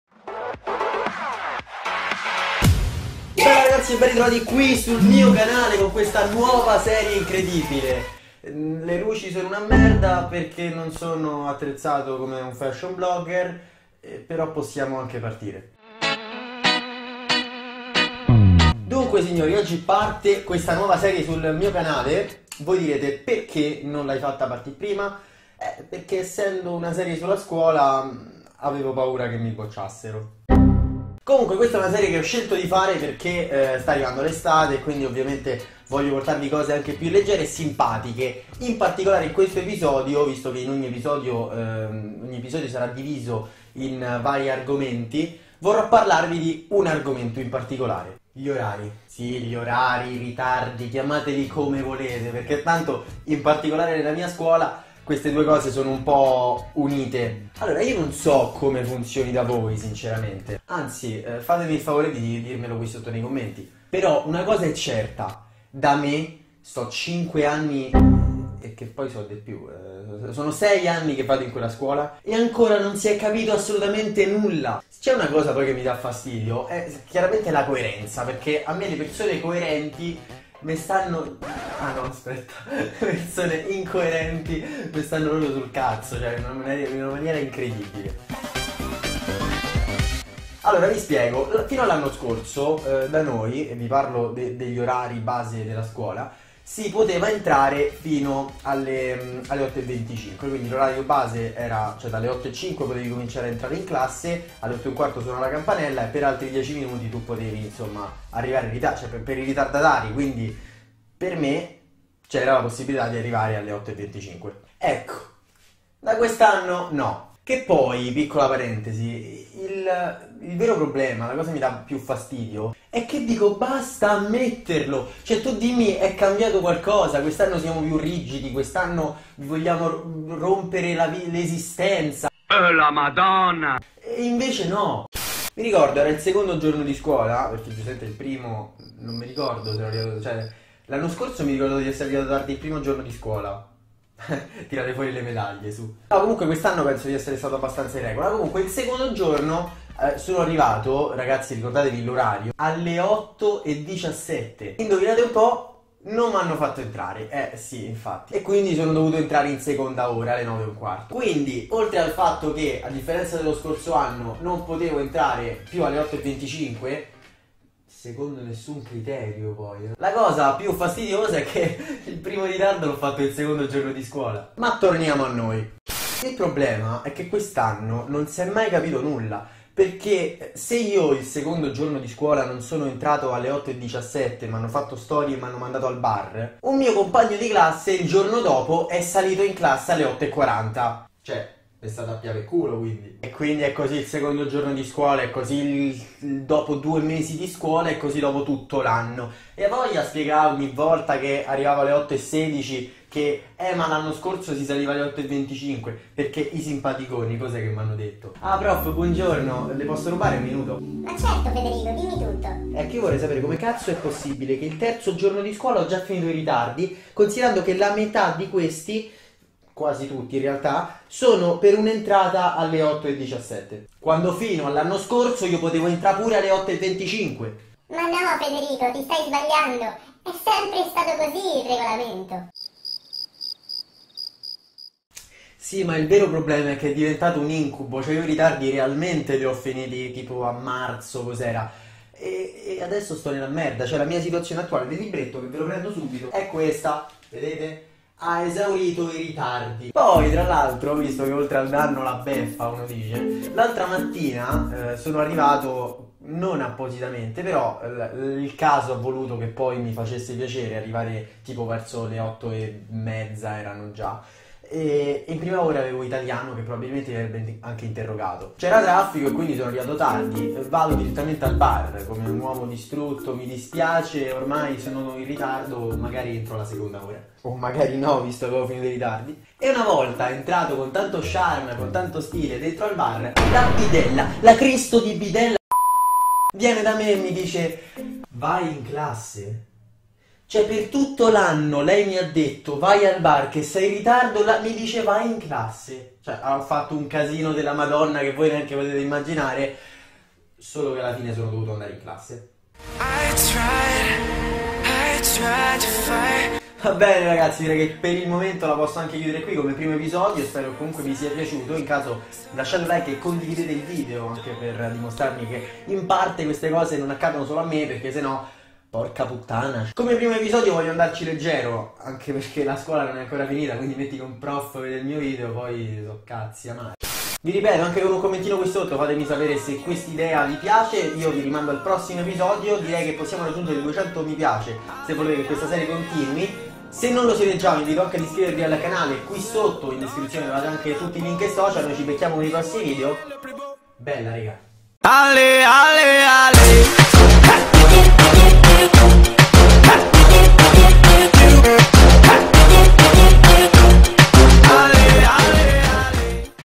"MAMMA, HO FINITO I RITARDI" - La SQUOLA fa SKIFO!! [NUOVA SERIE]
Bella ragazzi e benvenuti in questa nuova serie, in cui vi parlerò in generale della scuola (ovviamente in modo ironico) e di qualche mia avventura simpatica.

Kik - fedemarc99